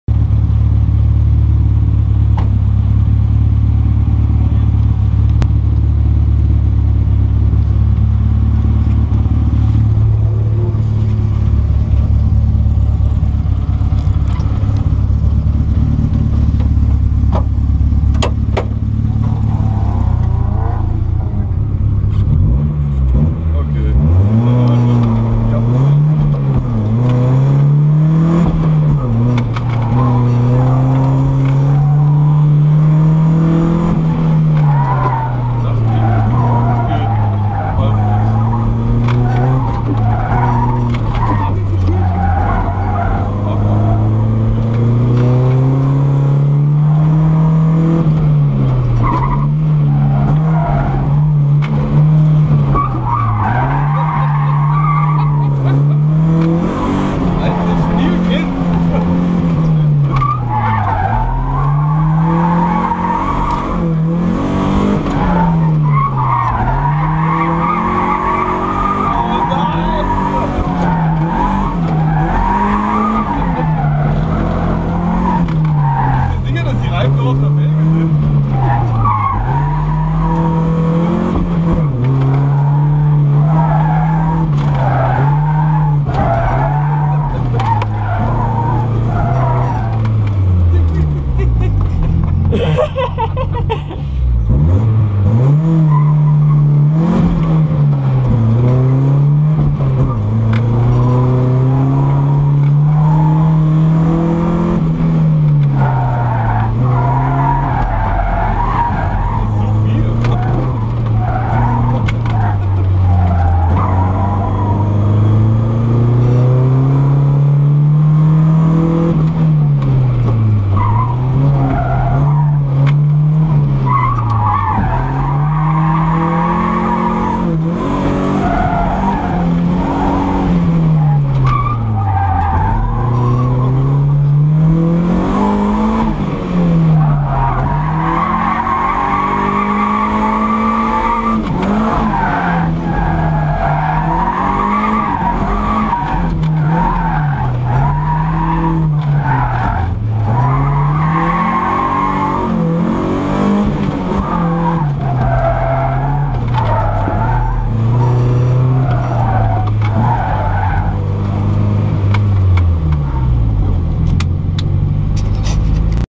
Bmw (e30) 318is - Drivers Eye - Onboard fun
Drove a friends Bmw 318is Rally car with my dad beside me. just had fun as hell driving this thing, not fast, but funny af! :D
Have fun and I hope you enjoy the video ;)